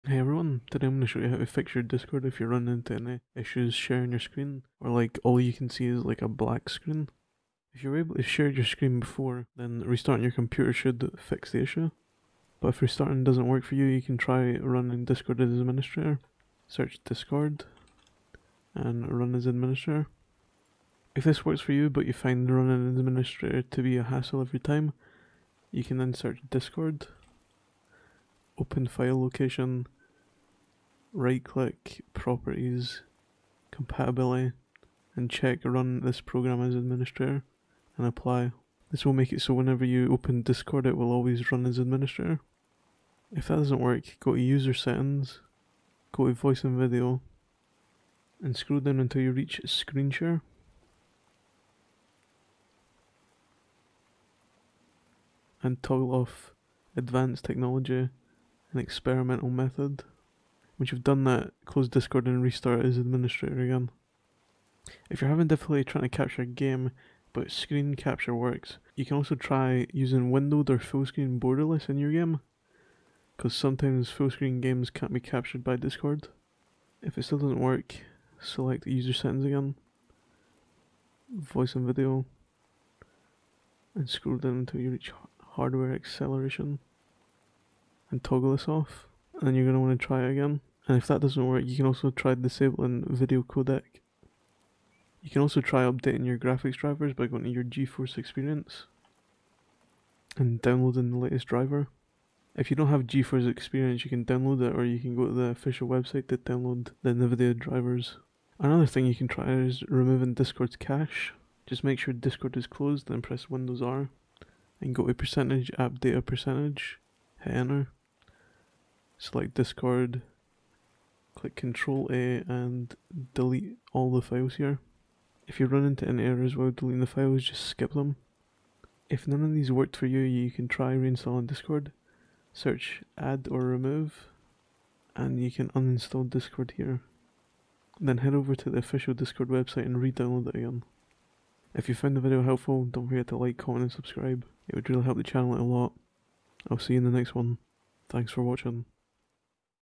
DISCORD - HOW TO FIX BLACK SCREEN/SCREENSHARE
COPY, PASTE:
[1] %appdata%

TEXT GUIDE:
[1] restart you computer
[2] Try running discord as administrator, you can also enable administrator by default, open discord file location, right click, properties and select compatibility, and enable run this program as administrator and click apply.
[3] open discord, select user settings, voice and video, scroll till you reach screenshare and toggle off advanced technology and experimental method, close and restart with admin.
[4] in your game ensure that its using windowed of fullscreen borderless, as sometimes discord cant capture fullscreen games.
[5] open discord, select user settings, voice and video, find hardware acceleration and turn this off. if that doesnt work try disabling the video codec directly above that aswell.
[6] update your graphics drivers, open geforce experience or go to the nvidias official website and download your graphics driver.
[7] press windows key + R, type %appdata%, select discord and delete everything in the file. if there are errors while removing files, skip them.
[8] if none of these worked for you, uninstall and reinstall discord.